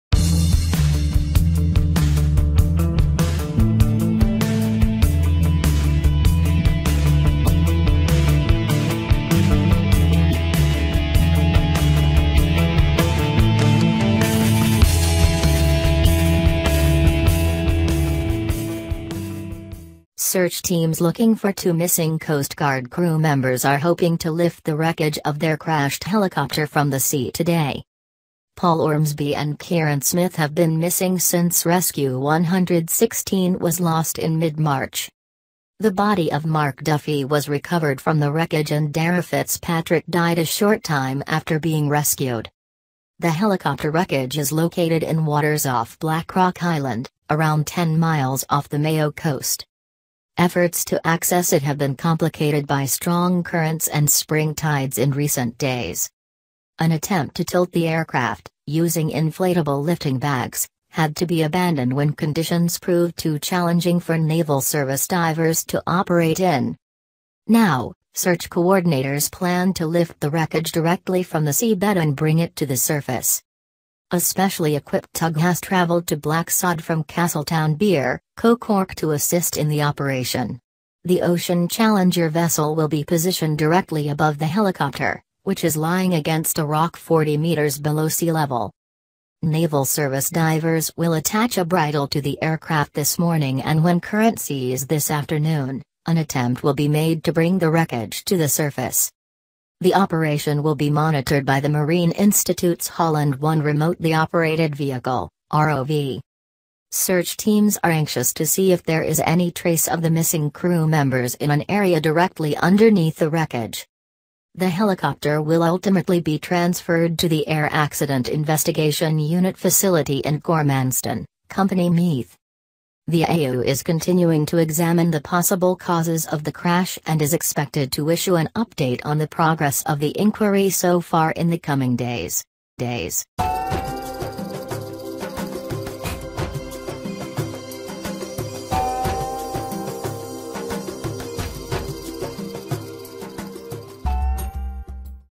Search teams prepared to lift wreckage of crashed helicopter Rescue 116
SH news_Search teams looking for two missing Coast Guard crew members are hoping to lift the wreckage of their crashed helicopter from the sea today.

Paul Ormsby and Ciarán Smith have been missing since Rescue 116 was lost in mid-March.

The body of Mark Duffy was recovered from the wreckage and Dara Fitzpatrick died a short time after being rescued.

The helicopter wreckage is located in waters off Blackrock island, around 10 miles off the Mayo coast.